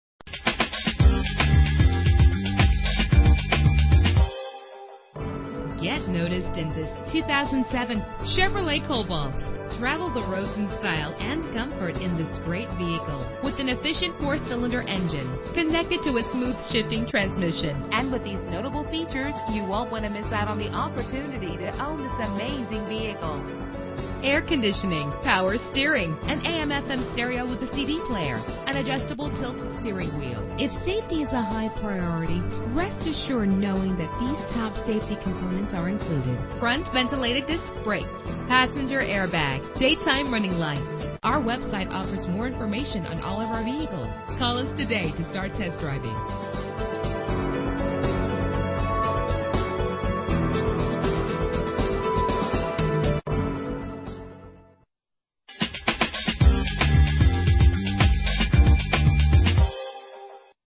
2007 Chevrolet Cobalt - Indianapolis IN
Year: 2007
Make: Chevrolet
Model: Cobalt
Trim: LS
Engine: 2.2 liter inline 4 cylinder 16 valve
Transmission:
Color: Silver
Mileage: 44959
Address:
4505 W 96th Street 96